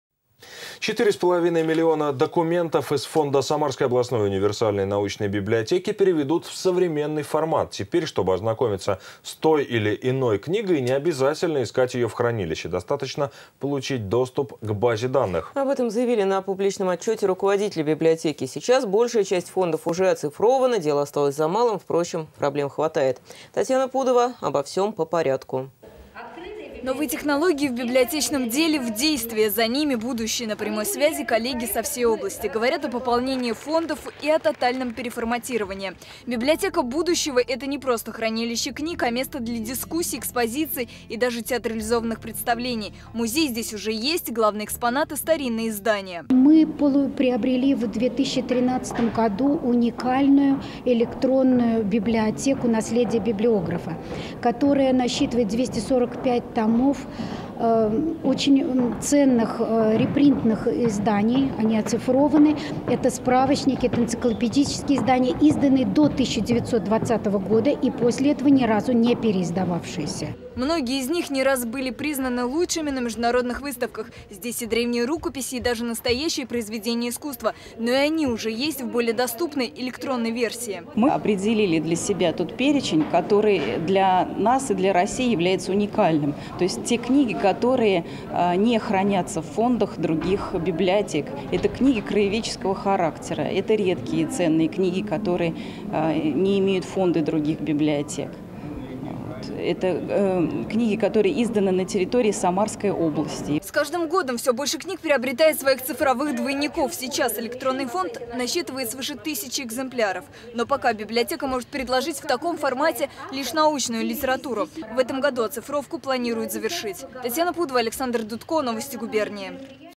Оцифровка фонда самарской областной универсальной библиотеки
4 с половиной миллиона документов из фонда самарской областной универсальной научной библиотеки переведут в Современный формат. Теперь, чтобы ознакомится с той или иной книгой, необязательно искать ее в хранилище, достаточно получить доступ к базе данных. Об этом заявили на публичном отчете руководители библиотеки. Сейчас большая часть фондов уже оцифрована, дело осталось за малым, впрочем, проблем хватает.